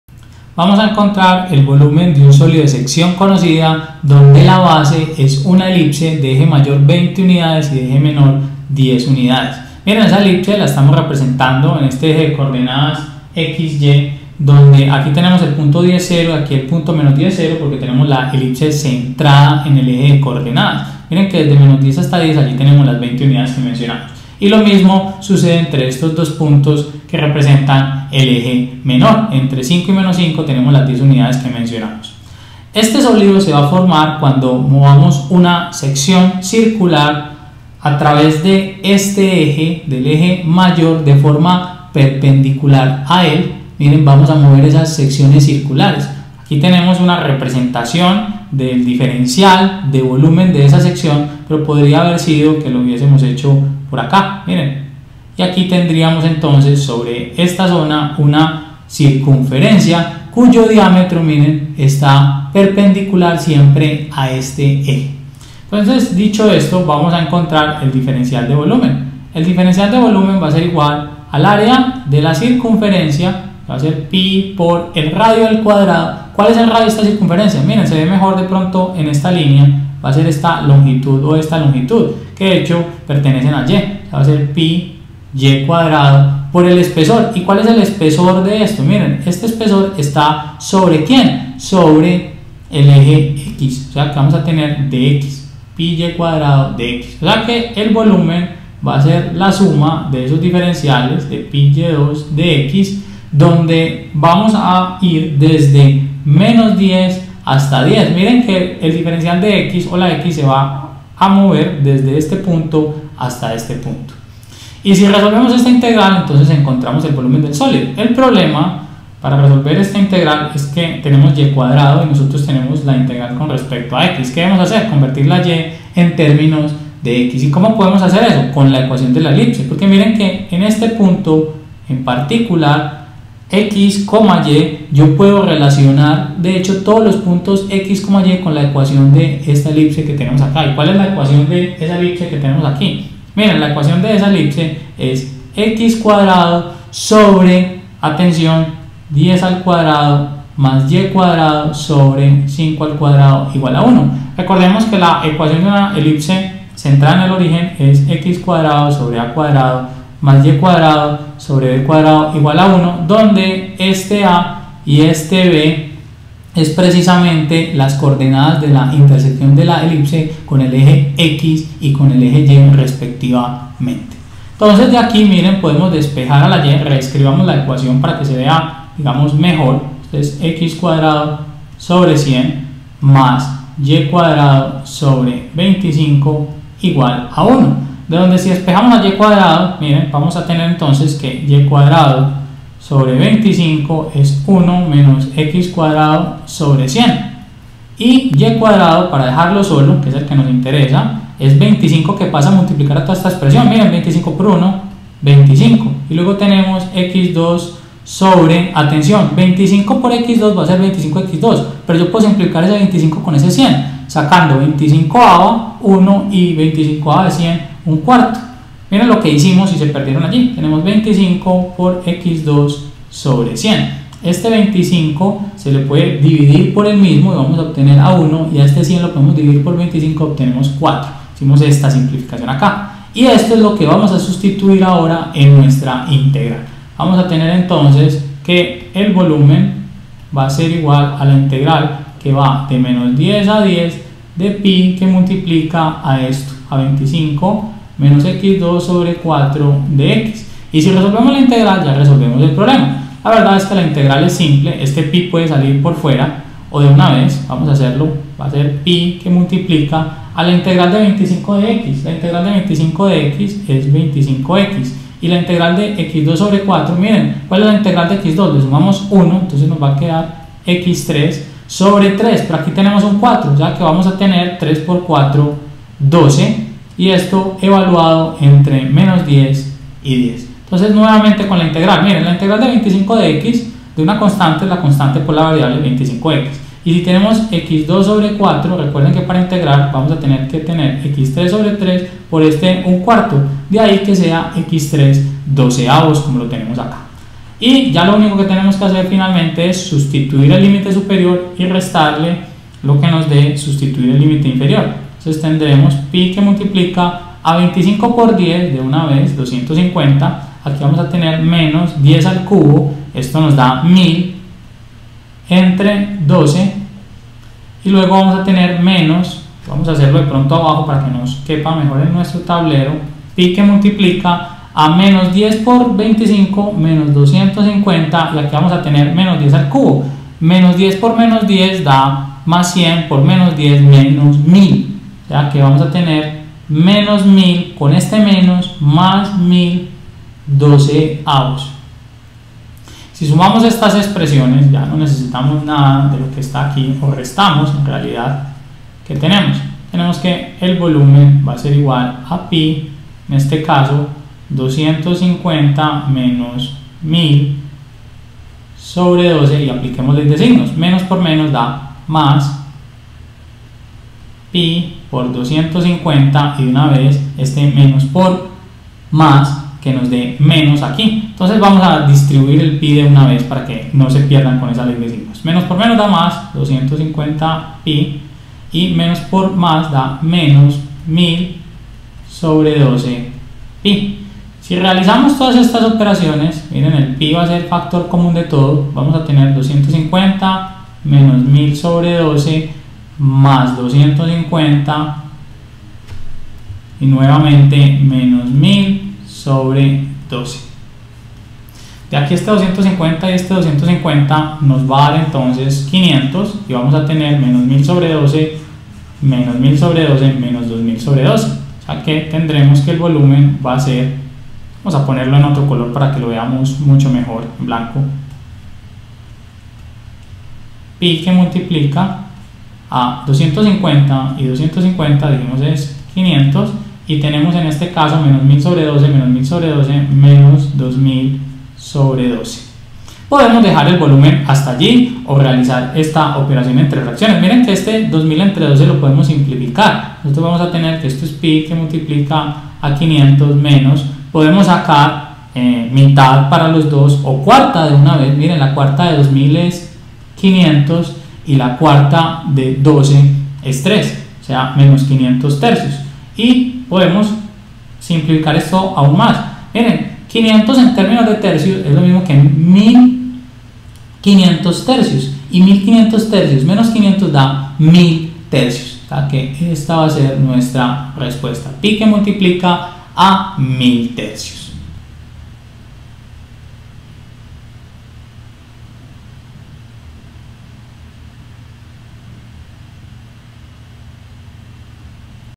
Volumen de un sólido sección conocida ejemplo 5
Aplicaciones de la integral definida
Cálculo del volumen de un sólido de sección circular (elipsoide)
En esta serie de ejemplos sobre el cálculo del volumen de sólidos de sección conocida se encuentra el volumen de un elipsoide el cual se forma al tener como base del sólído una elipse y como sección una circunferencia que se mueve a lo largo del eje mayor de la elipse.